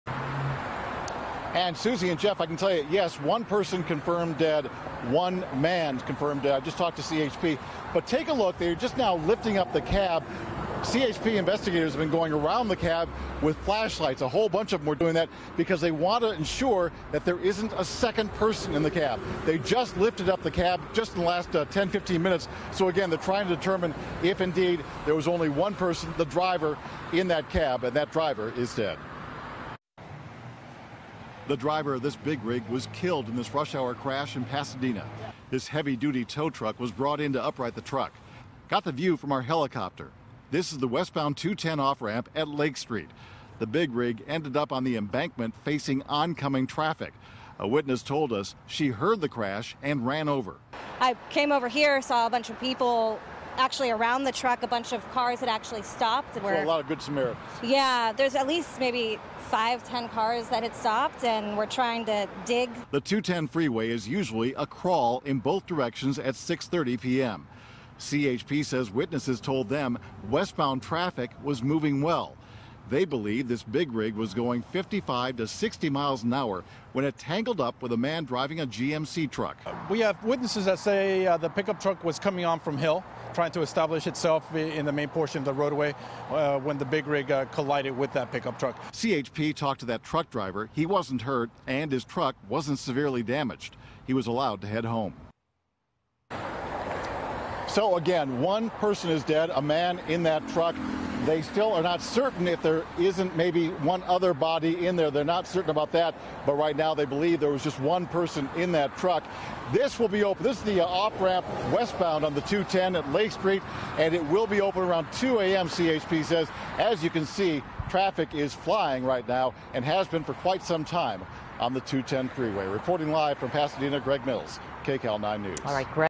Big Rig Overturns, Killing Driver
A big rig overturned on the 210 Freeway, killing one person and now traffic is moving once again. Greg Mills reports.